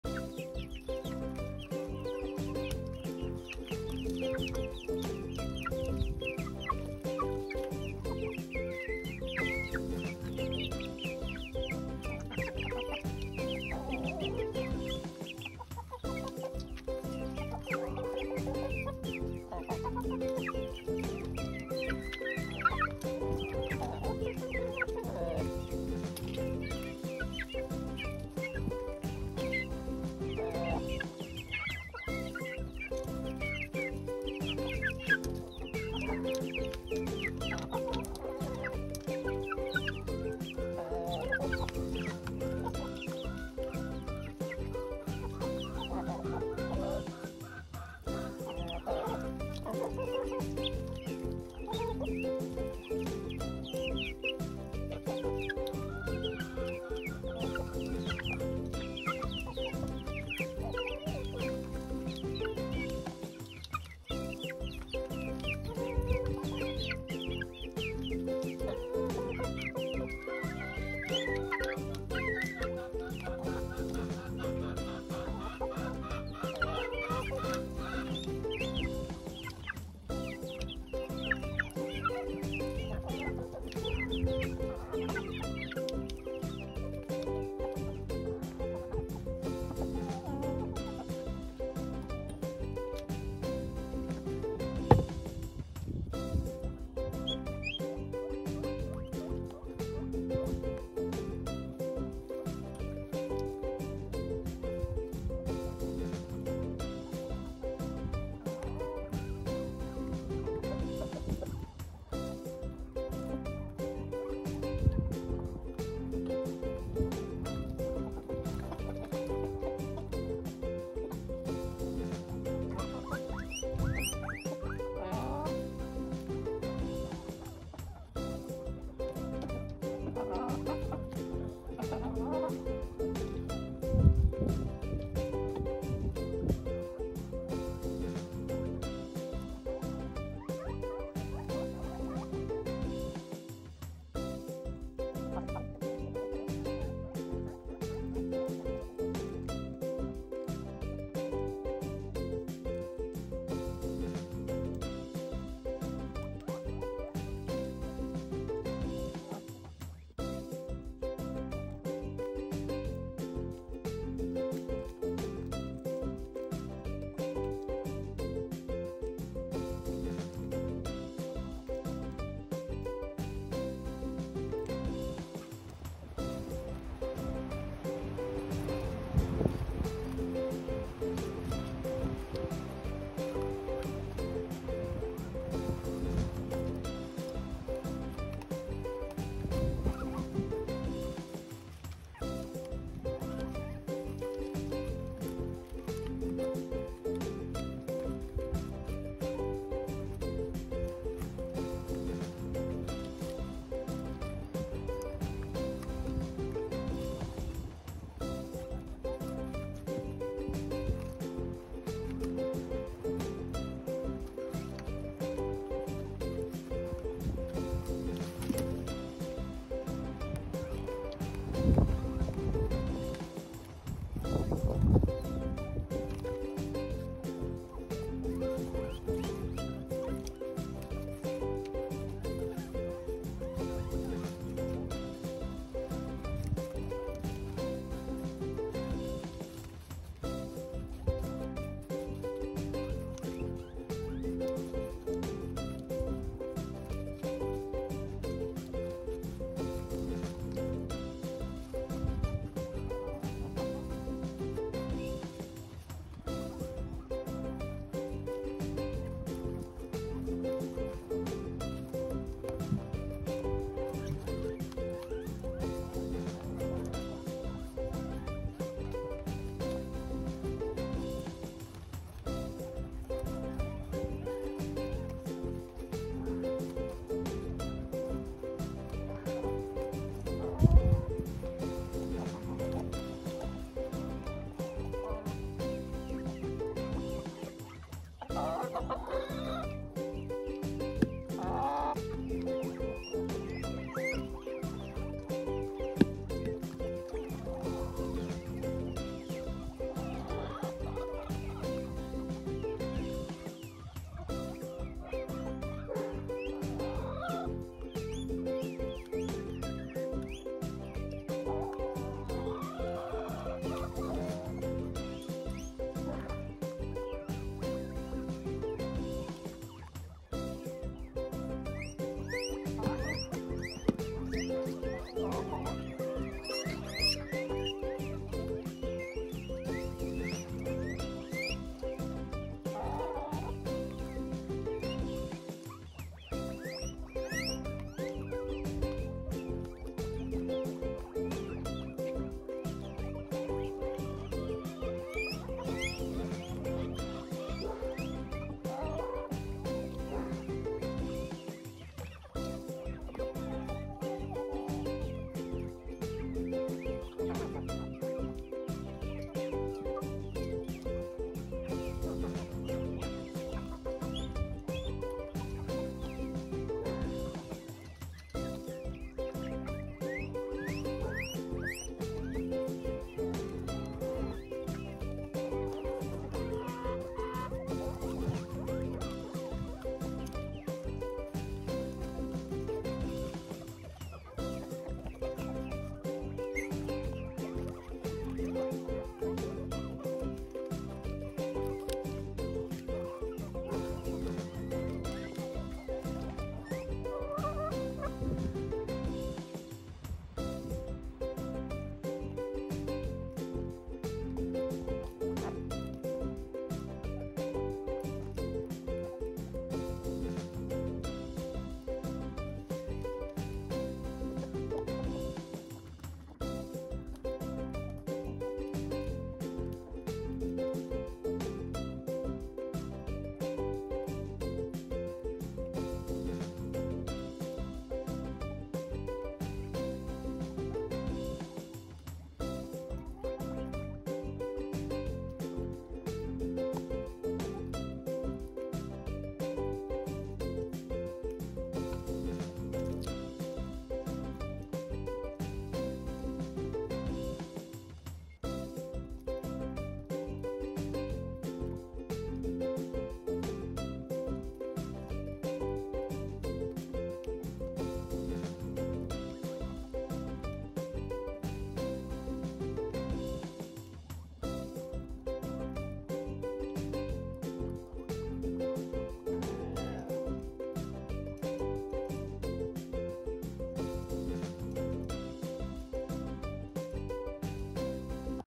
Cuys in Freilandhaltung mit den Hühnern zusammen
Ich baue mir gerade nach und nach eine Freilandhaltung auf mit den Hühnern zusammen. Noch ist es Alles im Werden und die Flächen sehen nicht so super aus, gerade im Winter. Der Rest vom alten Misthaufen wird noch geglättet und die Paletten werden anders gelegt als Verstecke. Im kleineren Gehege sind die Jungweibchen mit ein paar Müttern drin. Dort sind sie nur drinnen bis es ins große gehege geht oder in eine Zucht.